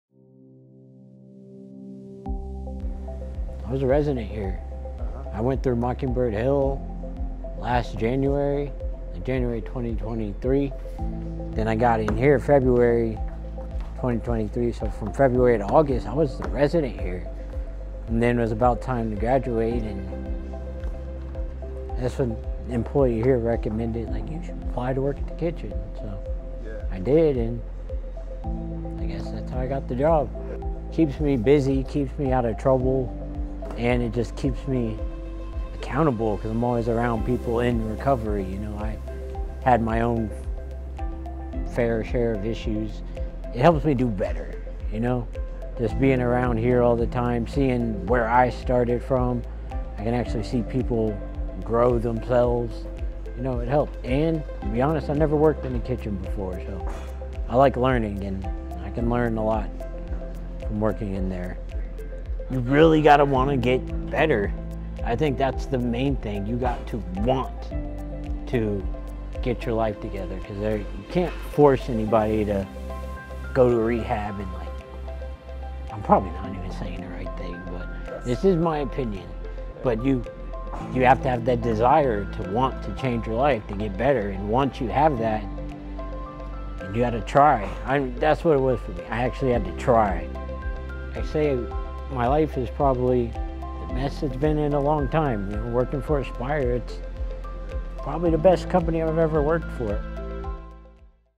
Employee Spotlight: Tim Maestas
A resident-turned-food service worker at Progress House, Tim Maestas continues to give back to the place that helped give him a fresh start and bring back his smile.